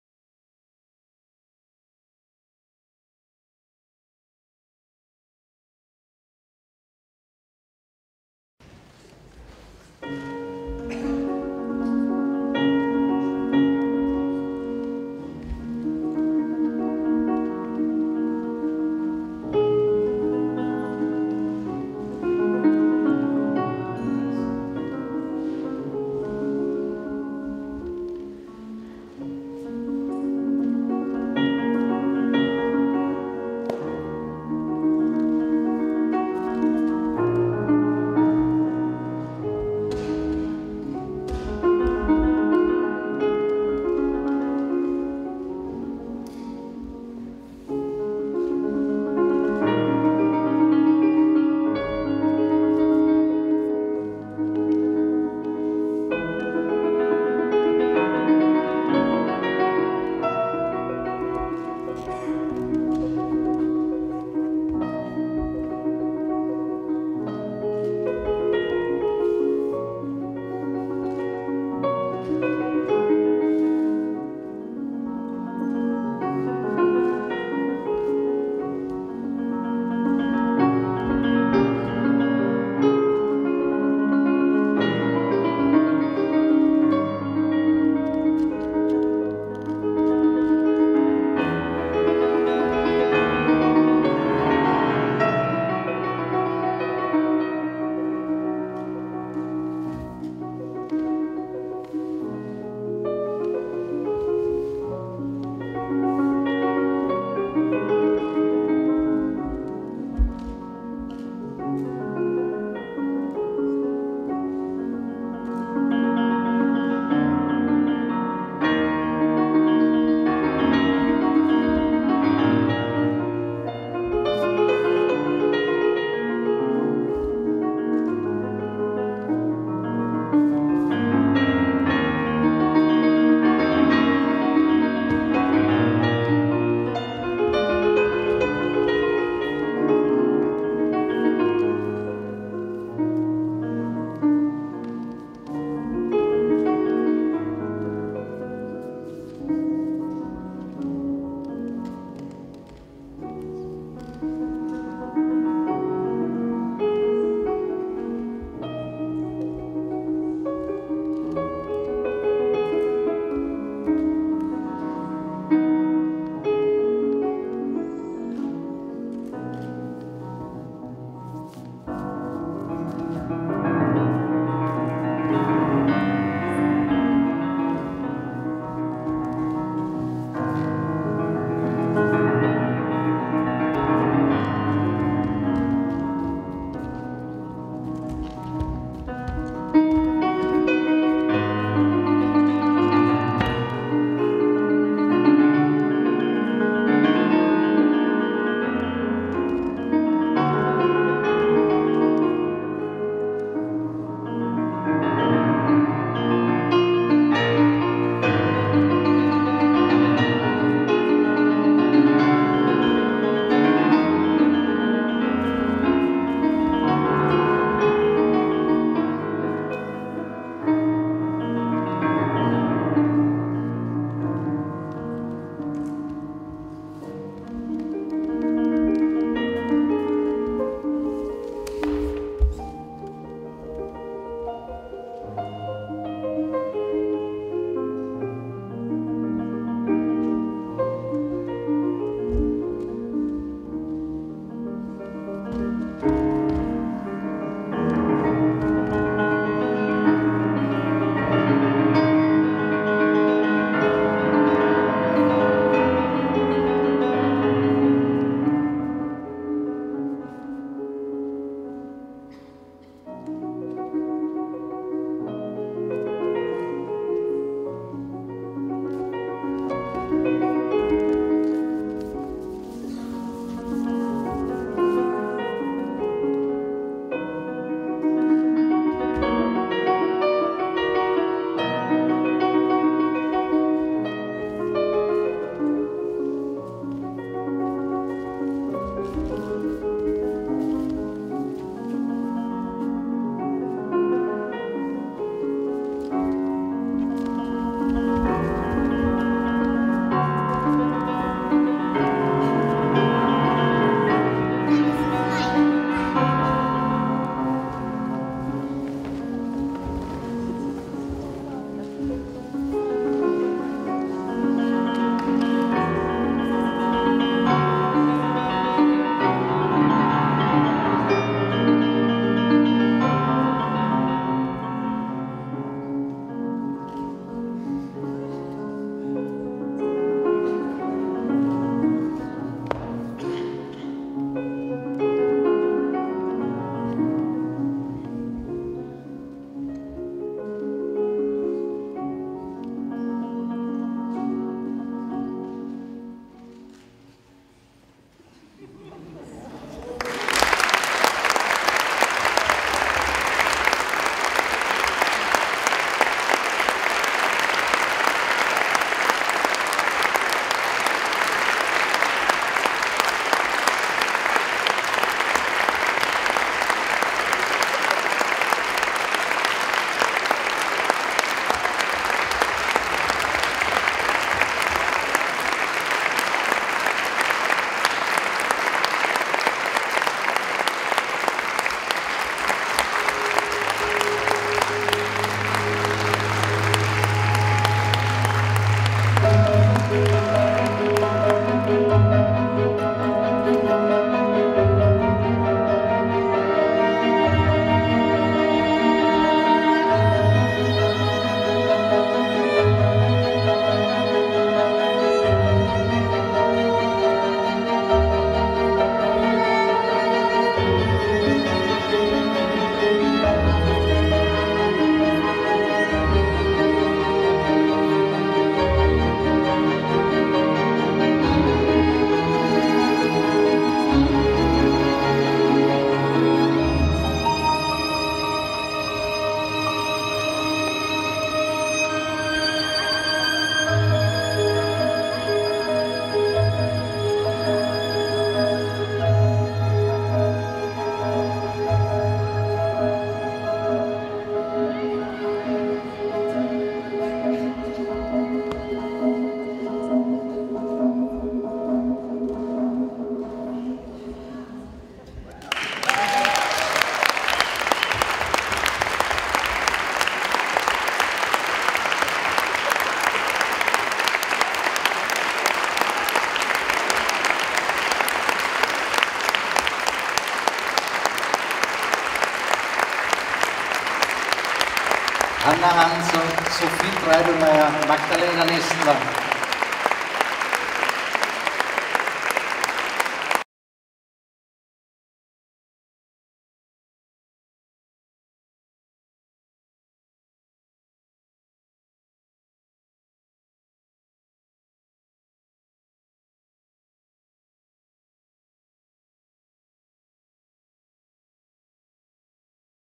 Ballett Tanzacademy Innsbruck, beimTangofestival Innsbruck 2012
Ballett Tanzacademy Innsbruck,
Magdalena Nestler, Sophie Treiblmayr, Hanna Hanser,
Choreographie Dagmar Kostolnikova
Show, Oct. 2012,
Tangofestival Innsbruck
Congress Innsbruck, Austria